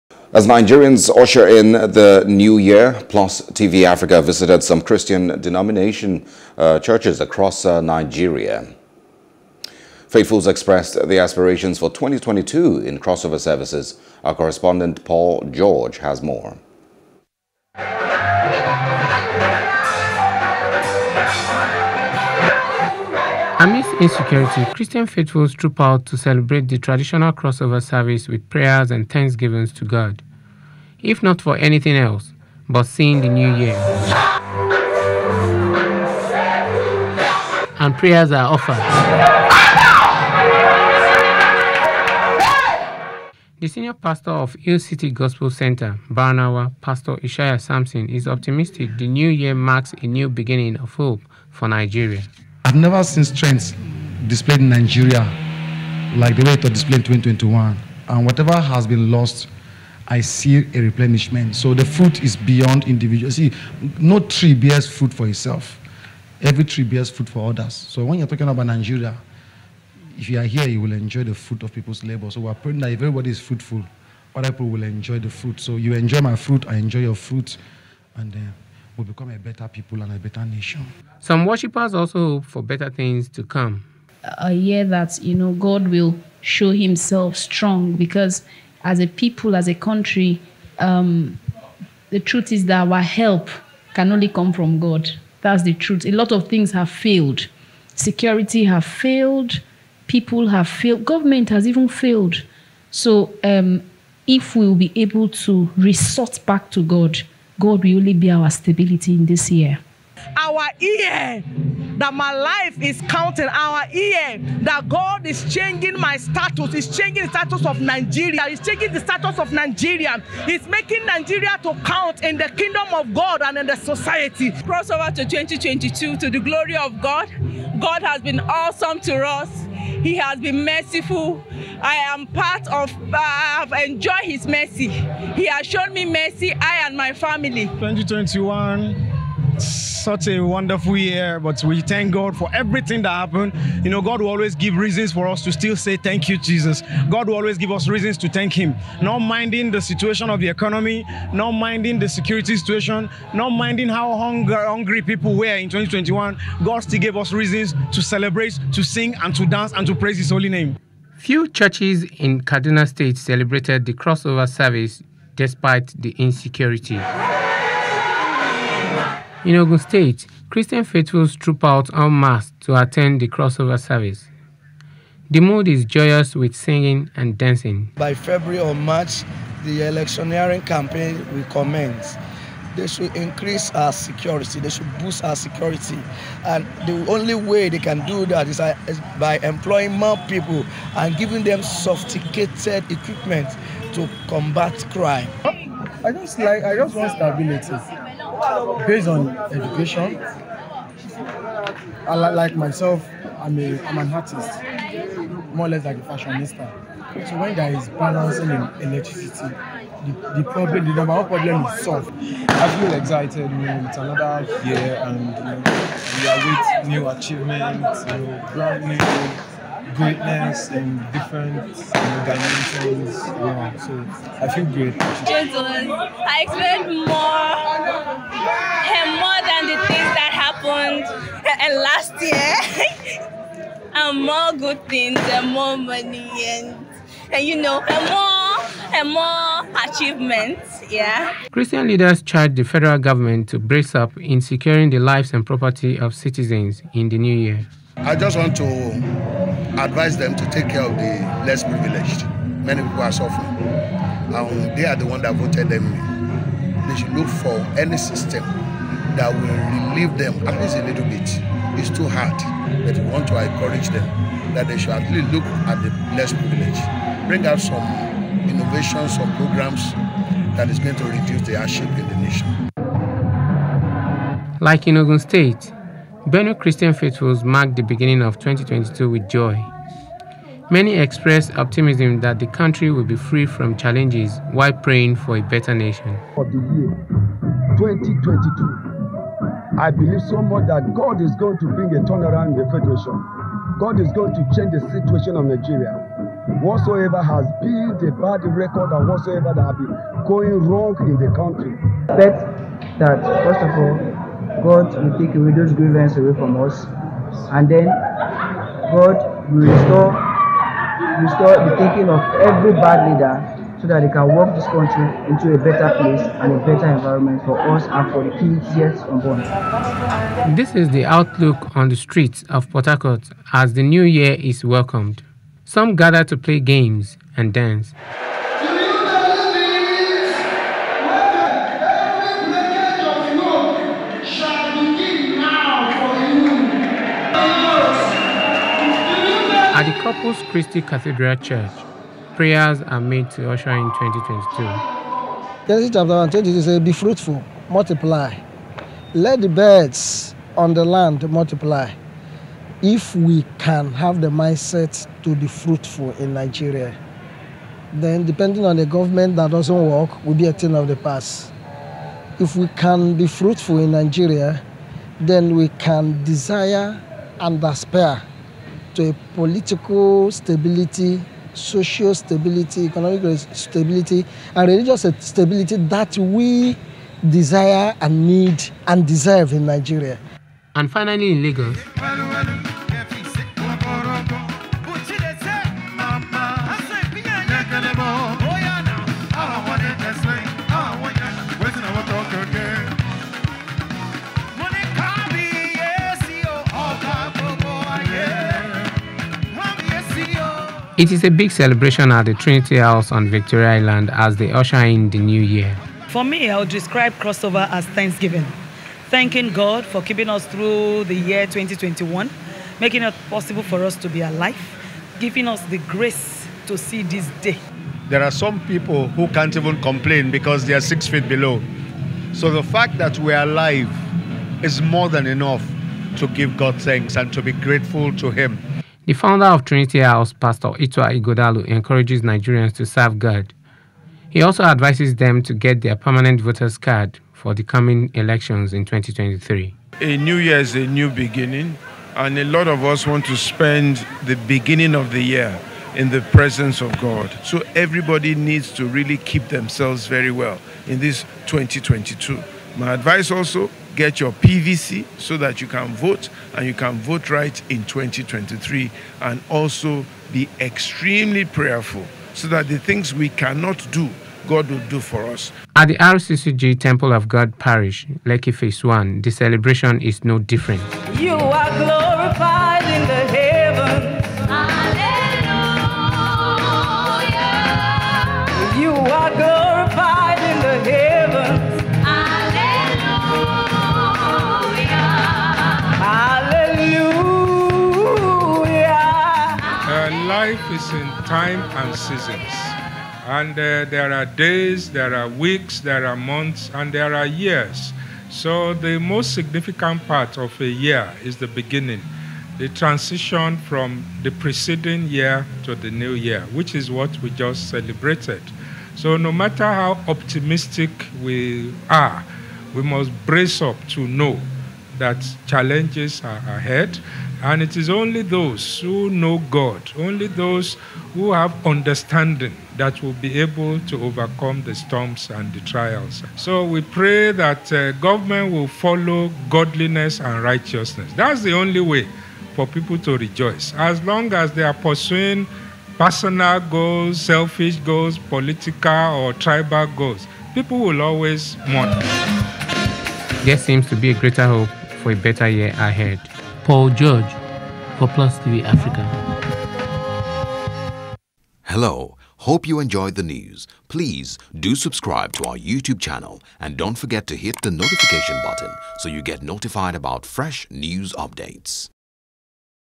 Christian Faithfuls Celebrate New Year Across Nigeria | NEWS
As Nigerians Usher In The New Year, Plus Tv Africa Visited Some Christian Denominations Across Nigeria.